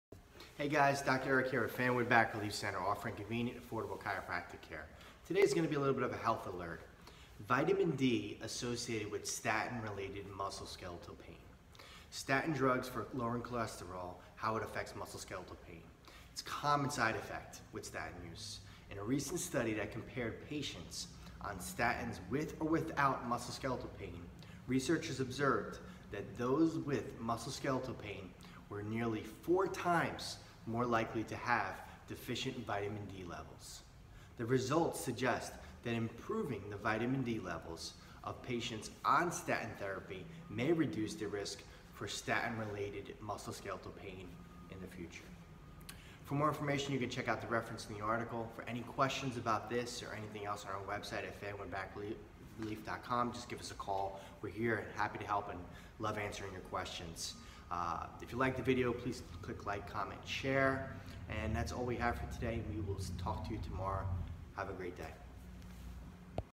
Vitamin D Associated with Statin-Related Musculoskeletal Pain | Fanwood Back Relief Center
Fanwood & Scotch Plains Chiropractor Eric Chludzinski of Fanwood Back Relief Center explains how Vitamin D is associated with statin-related musculoskeletal pain.

Back pain.
Neck pain.
Accident injury and whiplash.
Headaches.
Sciatica.

By:
Fanwood Back Relief Center